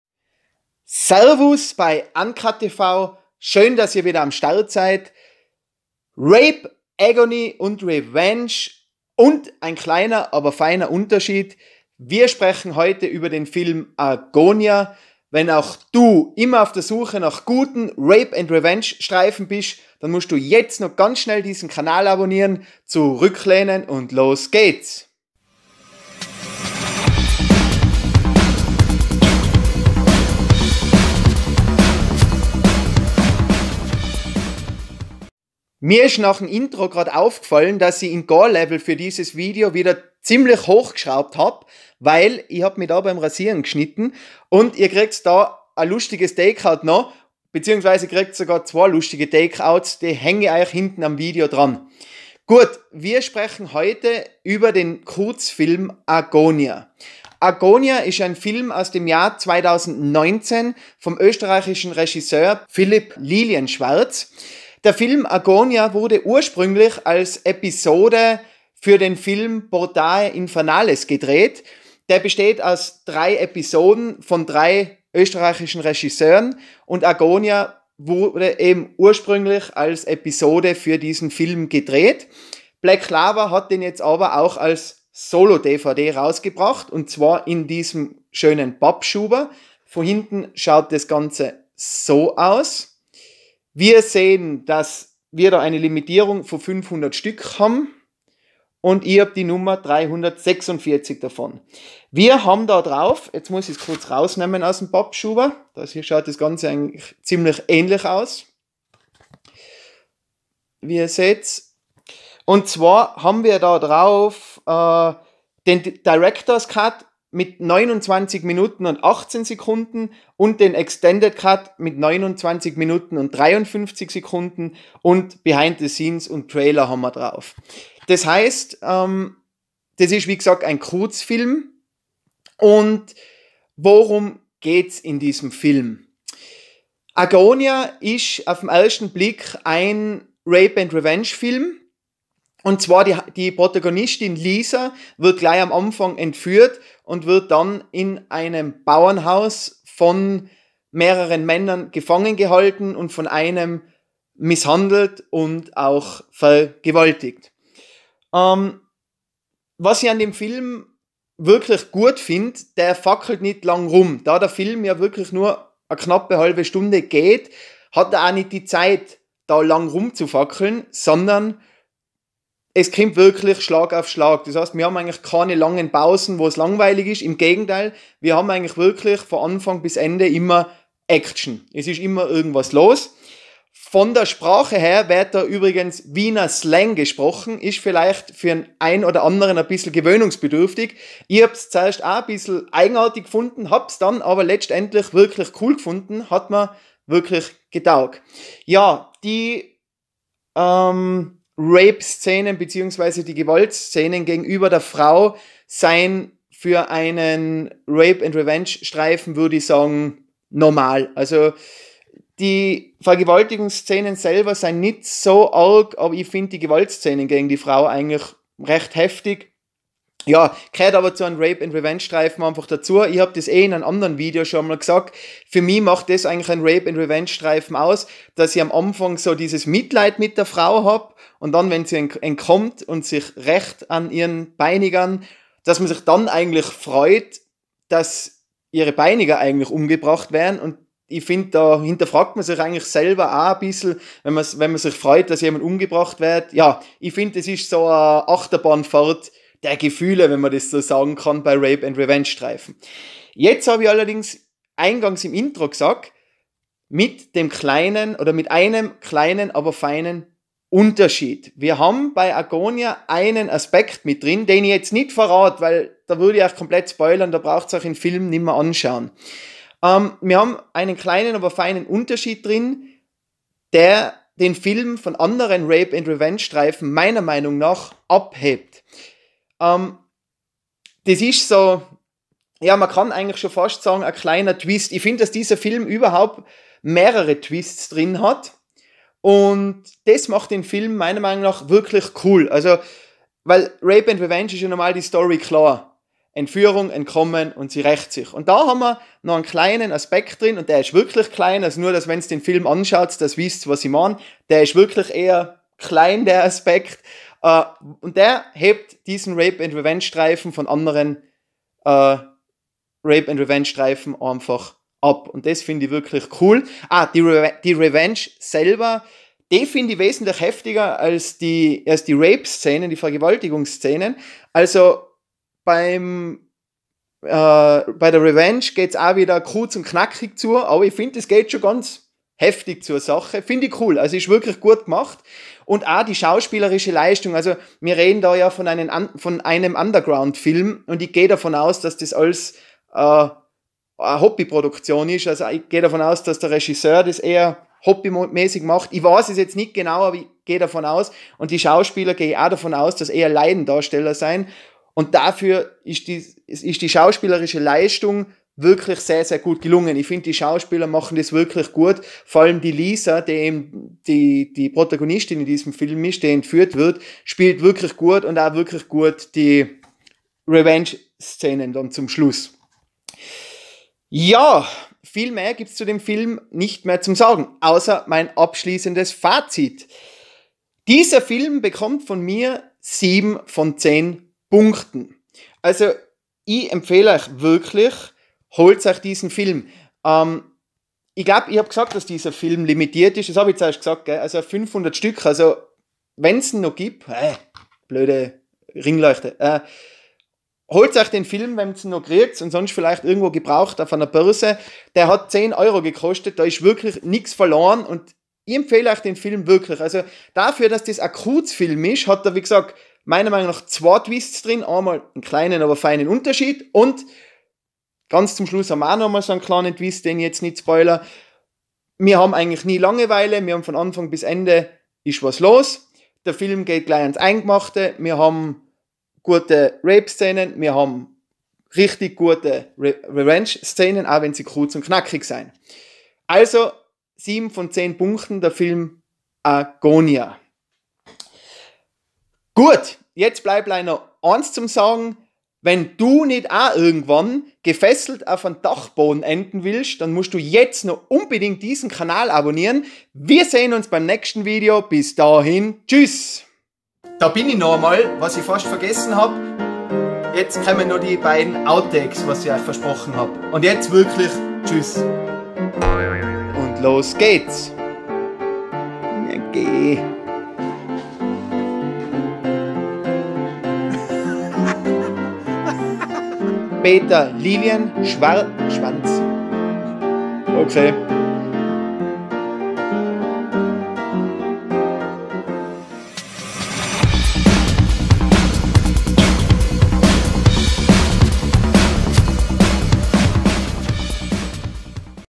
Review: AGONIA - Rape & Revenge vom österreichischen Regisseur Philip Lilienschwarz / Black Lava
Review zum österreichischen Rape & Revenge Kurzfilm Agonia vom österreichischen Regisseur Philip Lilienschwarz. Ursprünglich wurde Agonia als Teil des Films Portae Infernales, Drei Tore zur Hölle gedreht. Nun wurde der Film auf einer solo DVD von Black Lava Entertainment veröffentlicht.

00:00 Intro
00:52 Review
07:37 Outtakes